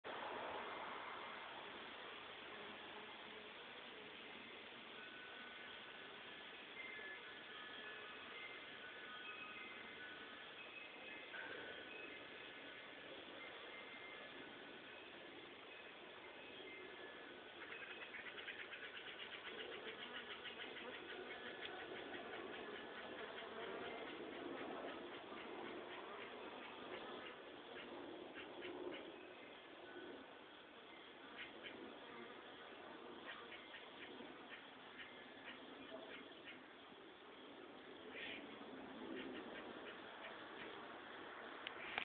Honey Bees Swarming My Hummingbird Feeder
In autumn, spurred on by cooler temperatures and dwindling flower sources of food ,  these honey bees swarm my hummingbird feeder.